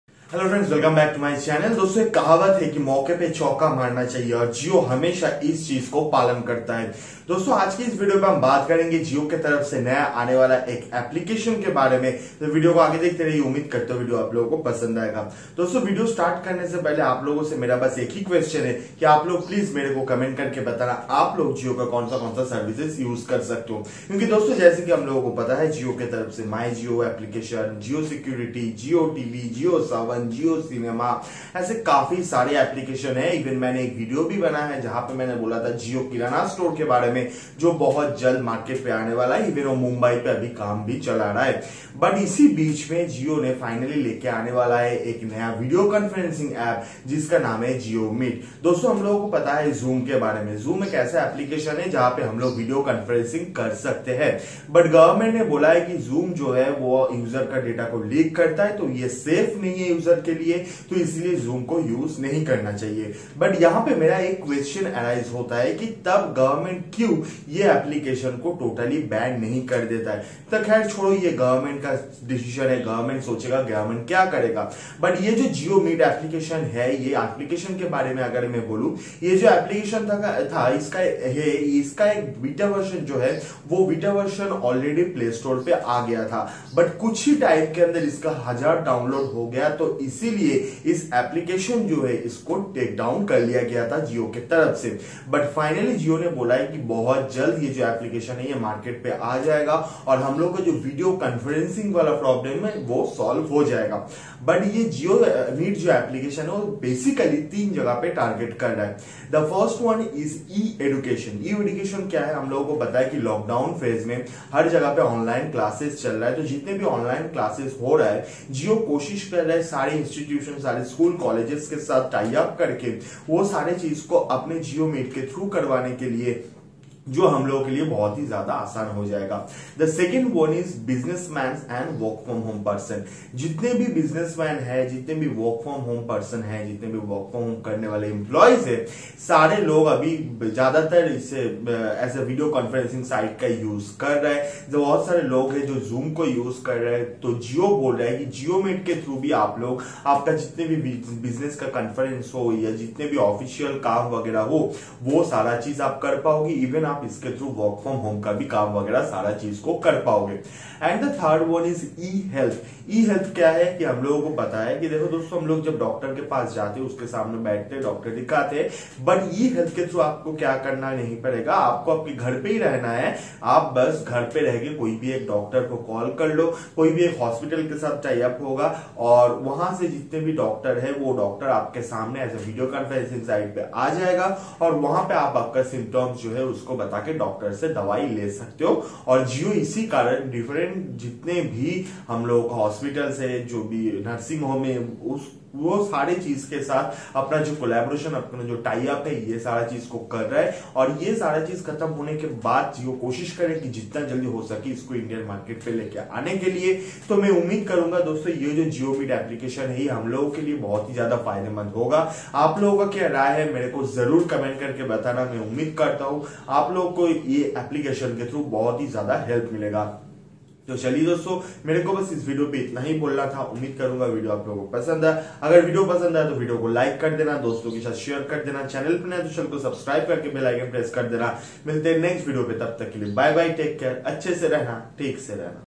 Jio Meet | Jio Video Conferencing App | Jio Meet vs Zoom App | How to use Jio Meet
Hello Friends,

This video is all about jio new video conferencing app Jio Meet. This app’s beta version listed in playstore but due to high download jio takedown from playstore. You can use this app in Android,Mac,Windows any plat form.

Video Captured & Edited By : Soumadwip Nandi